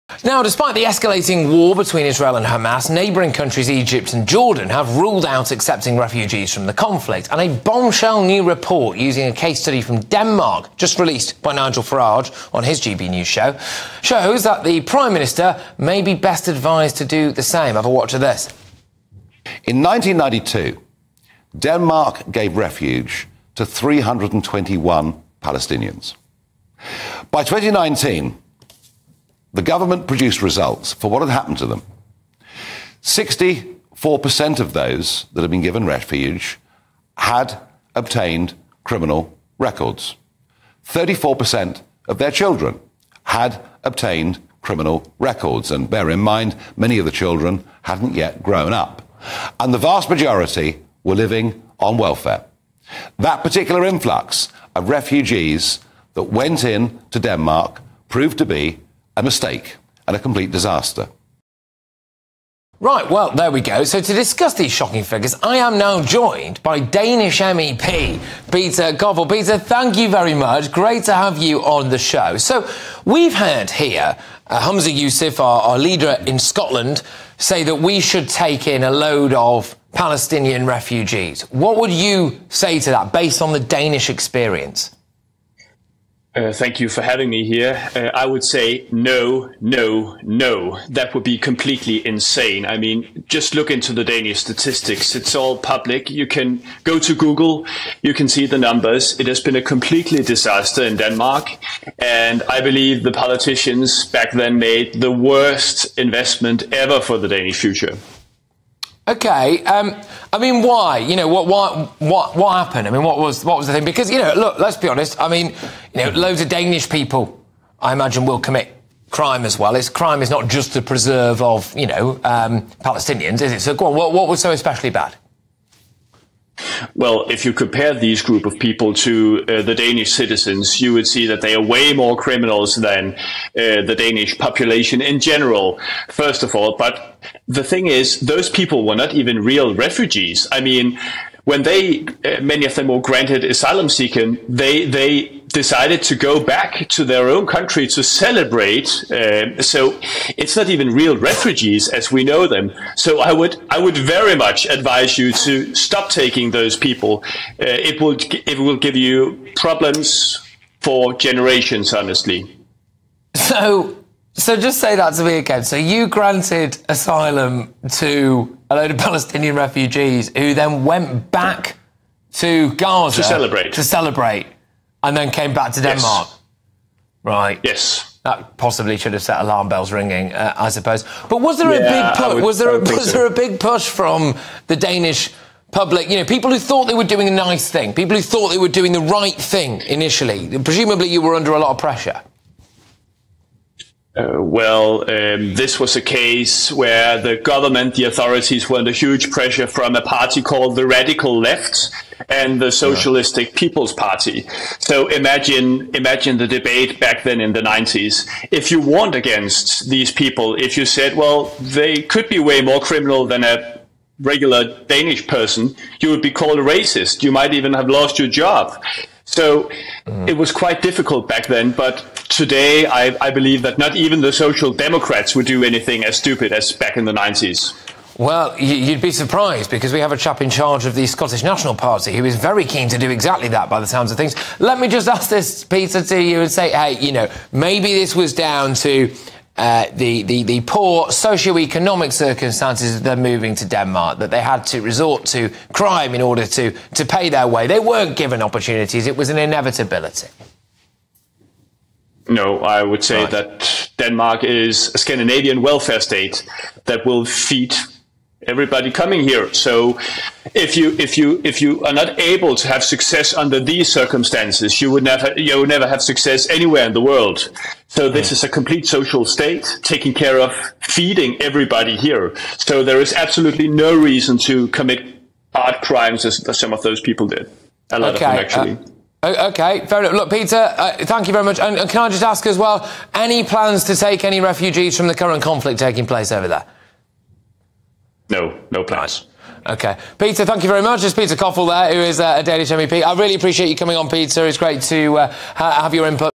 In 1992, Denmark took in 321 Palestinian refugees. What happened next was shocking.
64% of those Palestinians committed crimes in Denmark and 34% of their children (some of them still minors) are criminals. The vast majority of them are on welfare.

Ottawa wants to drastically increase Palestinian immigration to Canada. Do you think it's a good idea?